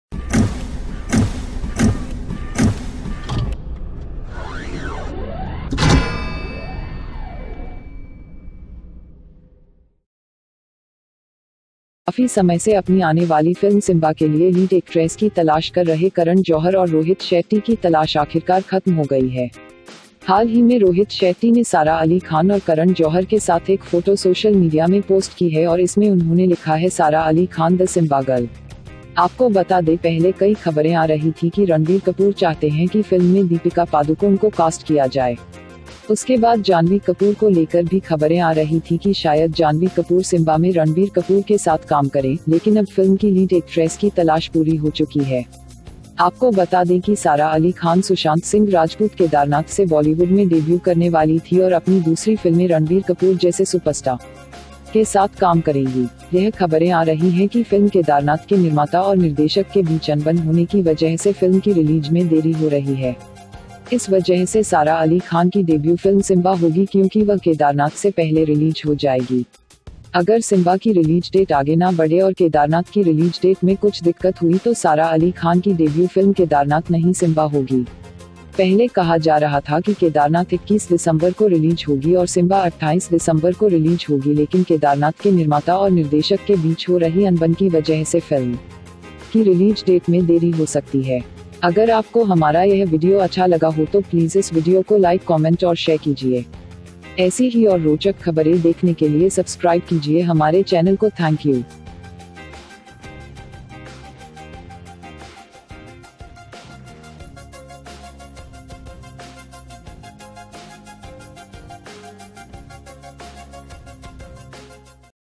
Heroin of Simmba movie | आखिरकार मिल ही गयी रोहित शेट्टी को नई हीरोइन सिम्बा के लिए ! Toffee media
Herein of  Simba movie |
आखिरकार मिल ही गयी रोहित शेट्टी को नई हीरोइन सिम्बा के लिए !

About : TOFFEE MEDIA is a YouTube Channel, where you will find technical videos, Entertaining New Video is Posted Everyday